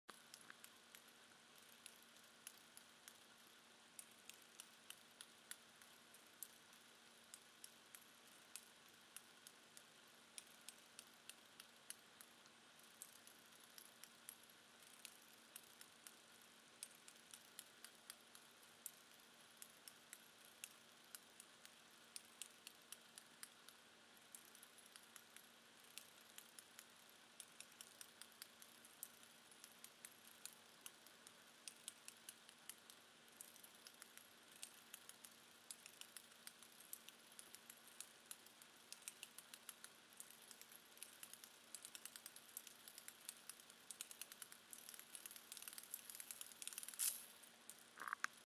Flip Dots - Brose - Arduino
First flip dot move with the brose controller and an Arduino